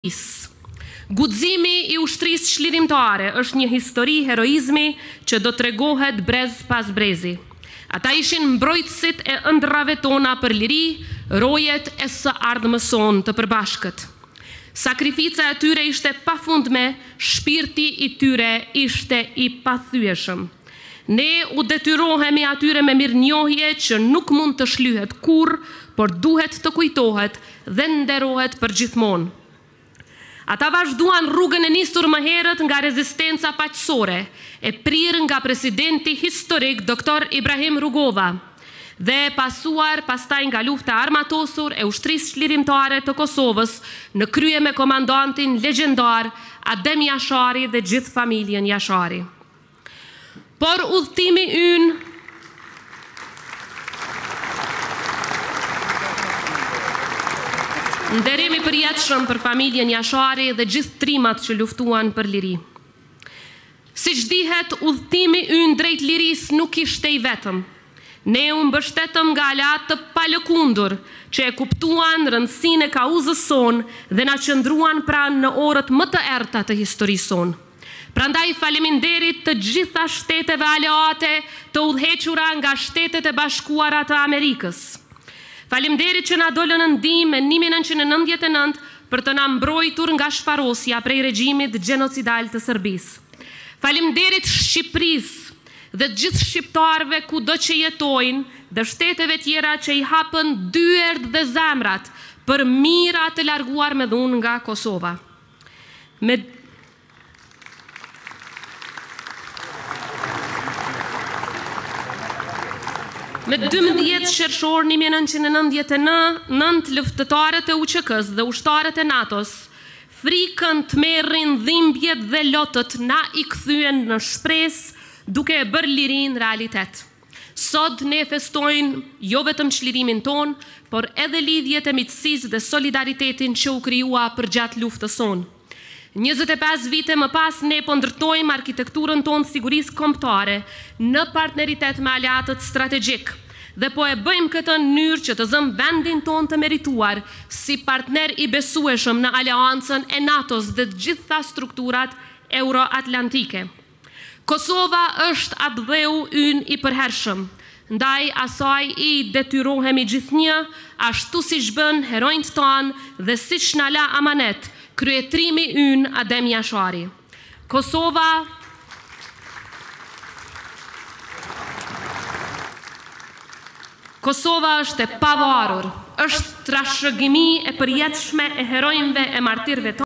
Presidentja Osmani: Faleminderit Shqipërisë dhe gjithë shqipëtarëve
Kosova Today nga e hëna në të premte me fillim nga ora 12:30 nën drejtimin e Jehona Bajramit dhe Kron Sadikut.